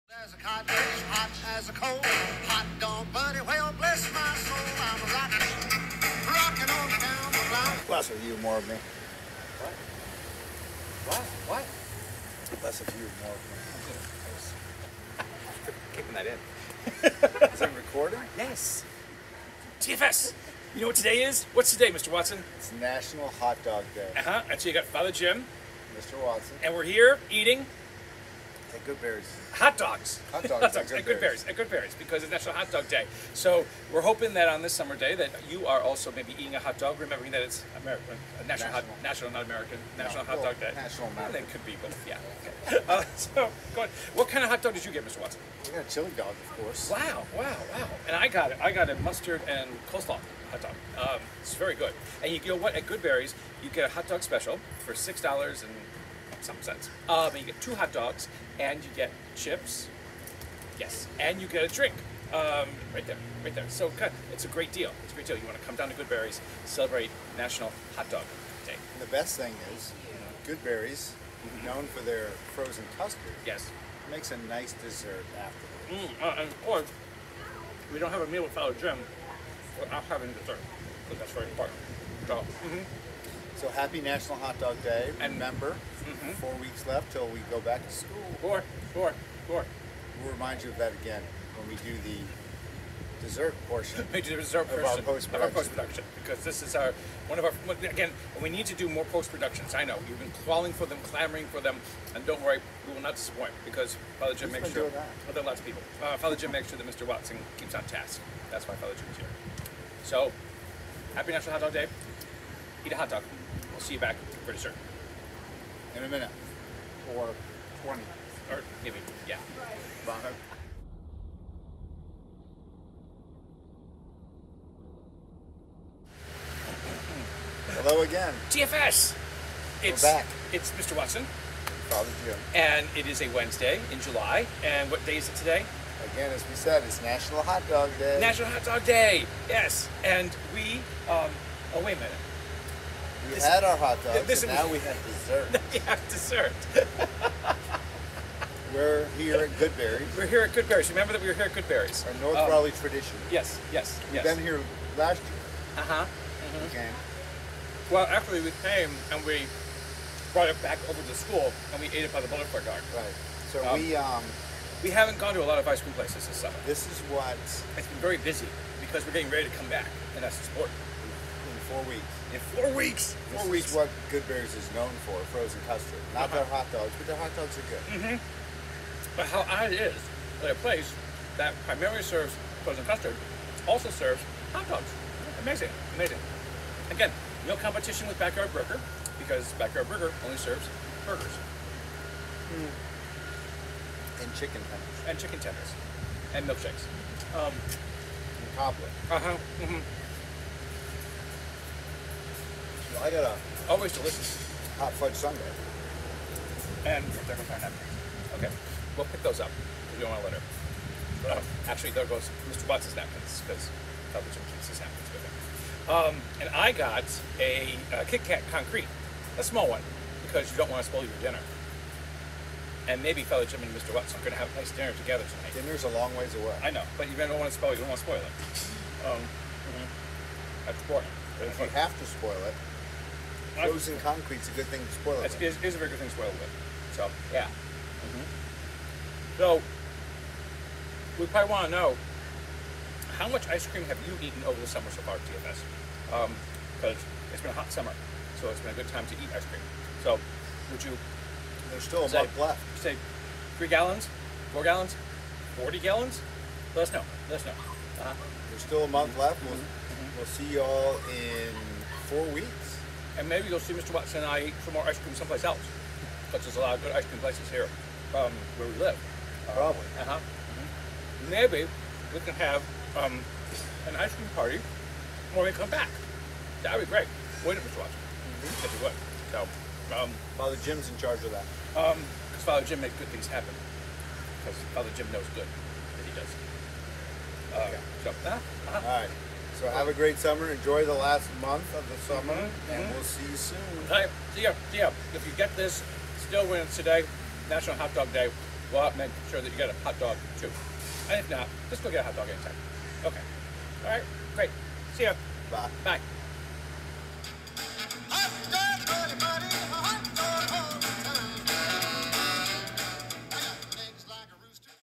Nat'l Hot Dog Day is July 21st!
Father Jim and Mr. Watson go to Goodberry's to celebrate National Hot Dog Day...and have a delicious frozen custard dessert too!!